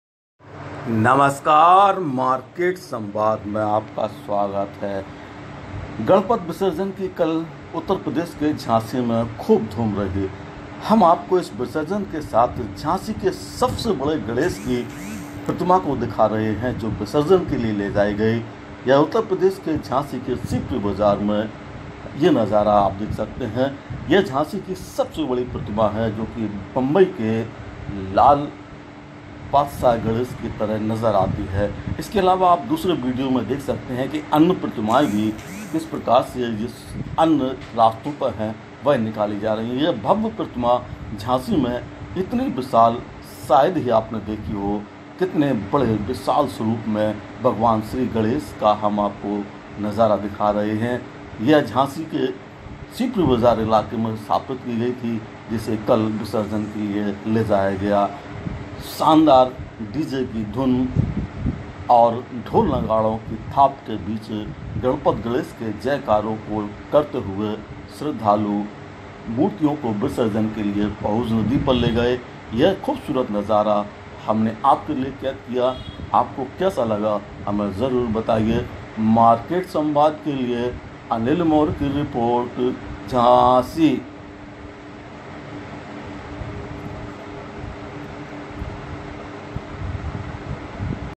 झांसी की सबसे बड़ी गणेश प्रतिमा, देखे कैसी है? रिपोर्ट-अनिल मौर्य
उत्तर प्रदेश के झांसी में कल भगवान गणेश जी की प्रतिमाओं का विसर्जन कार्यक्रम हुआ झांसी में कई स्थानों पर लगाए गए पंडाल में भगवान श्री गणेश की प्रतिमाओं को विराजमान किया गया था हम आपको  यात्रा में झांसी के सबसे बड़ी गणेश प्रतिमा के दर्शन करा रहे हैं